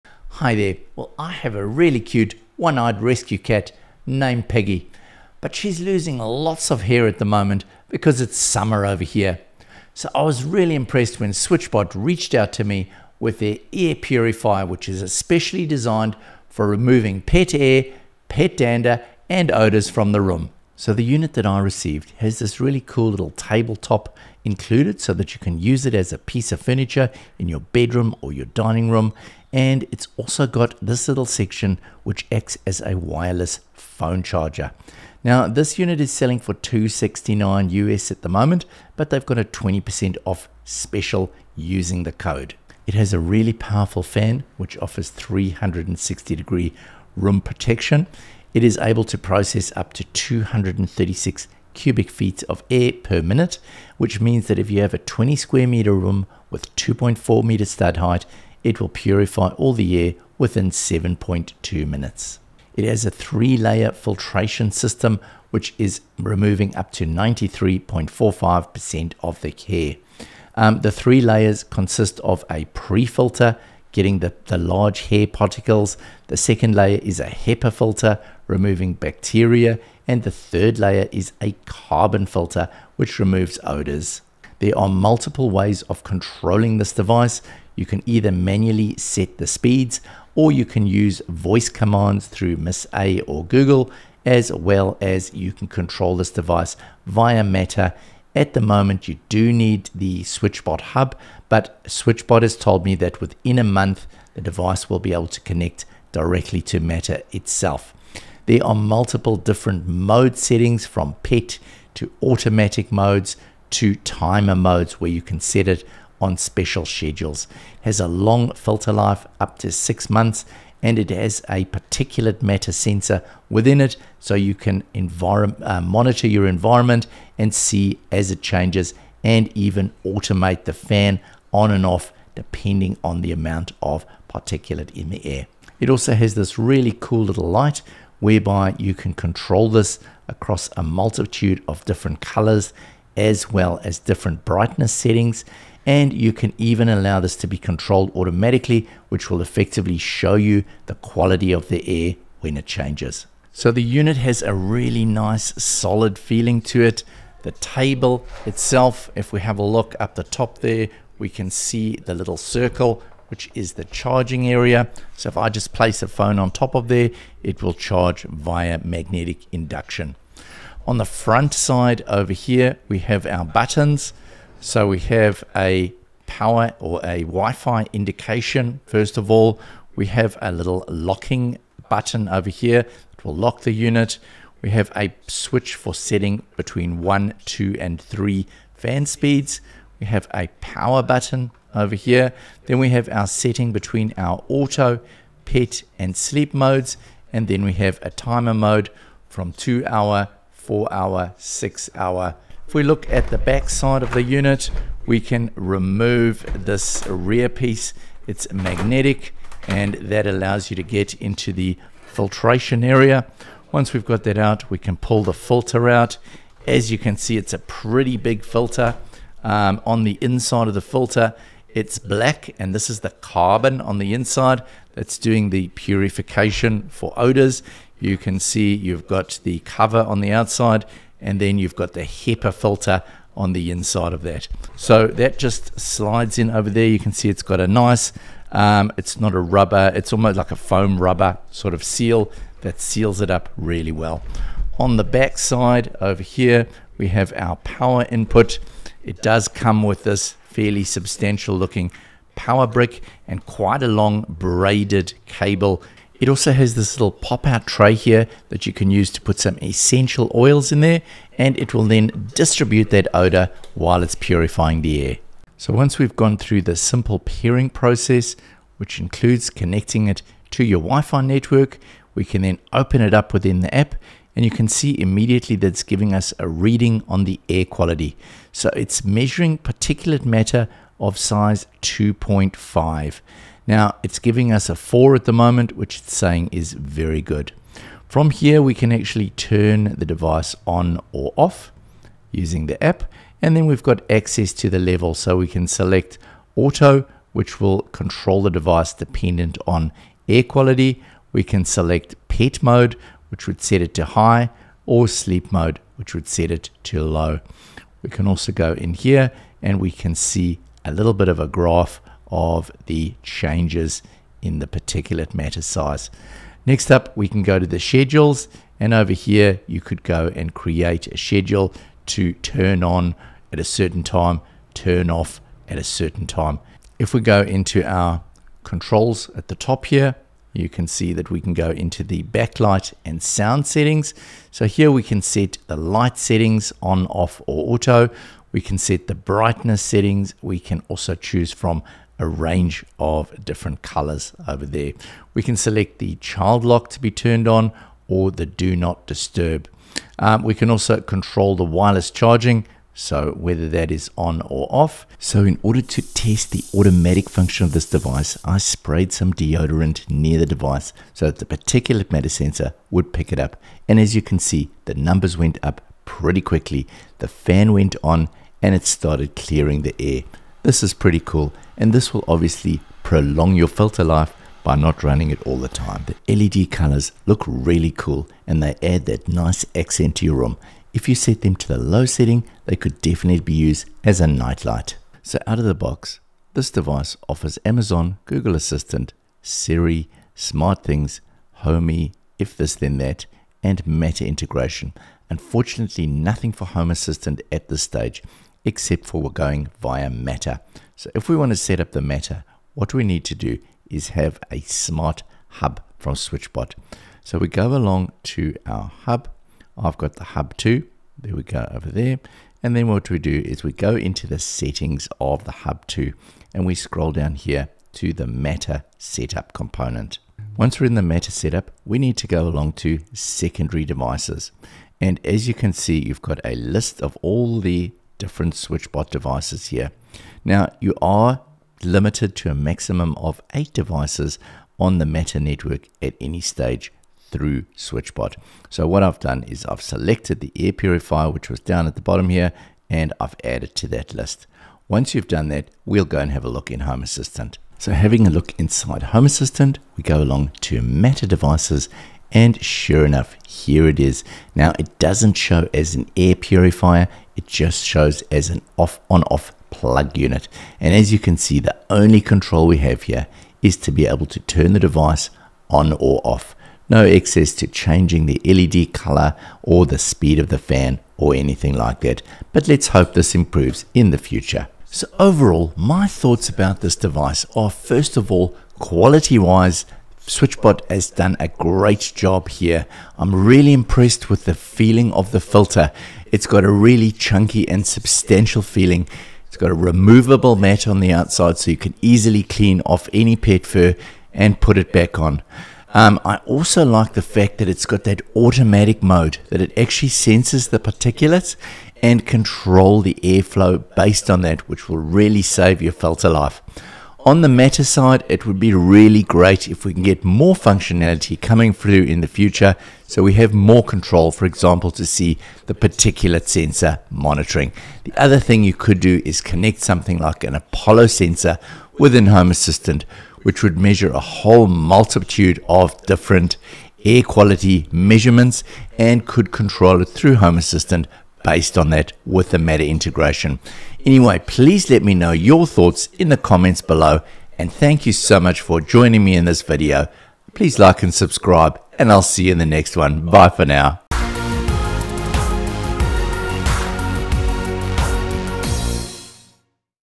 Switchbot has rescued me from cat hair with the Table Air Purifier!
Testing and review of the SwitchBot Air Purifier Table.  I also use Matter to integrate it with Home Assistant